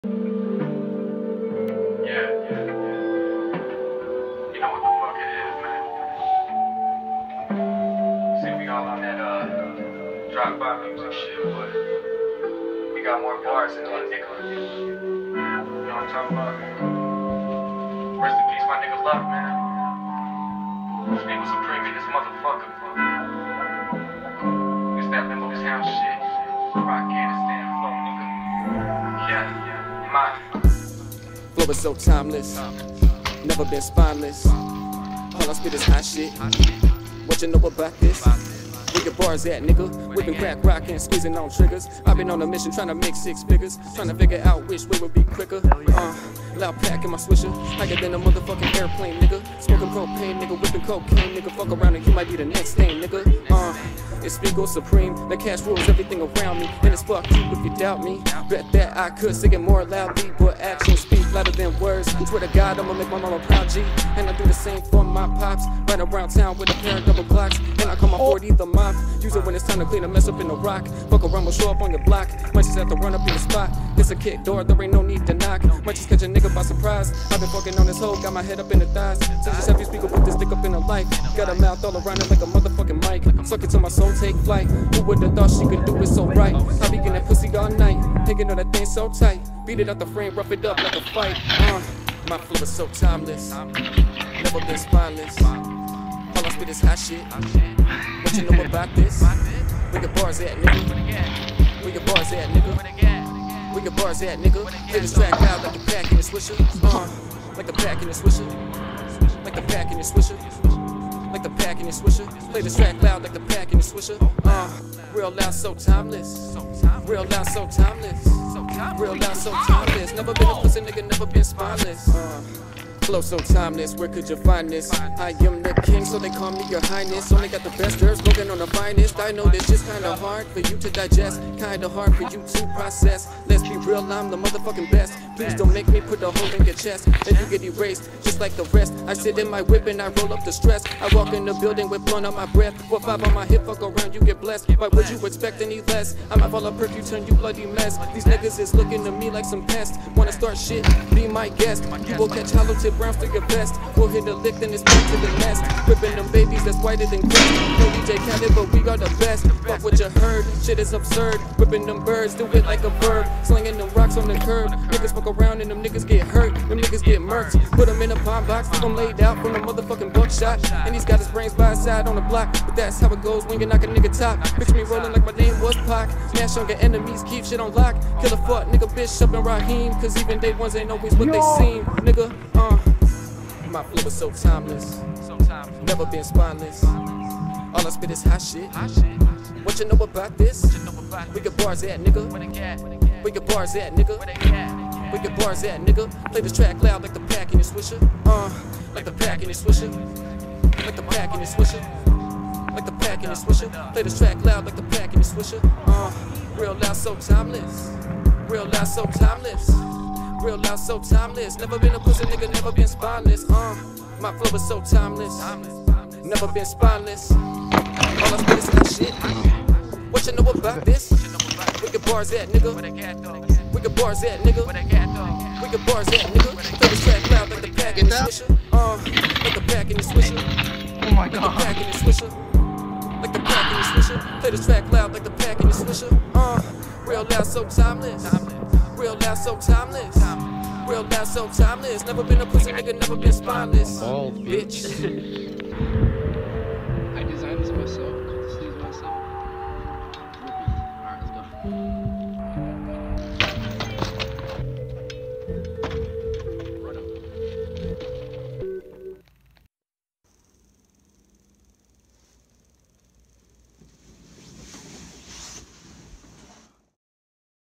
Rafael Martinez “Timeless” Bmx Edit
19 Year Old Rafael Martinez From Belvidere, Illinois Doing What He Loves Best Bmxing

I Had A Fun Time Editing This Video A Big Shout Out To My Friends For Filming And Coming With To Make This Edit Possible

Check Me Out On Instagram @rafaelbmx27

Check Him Out On SoundCloud “Spiegel Akira”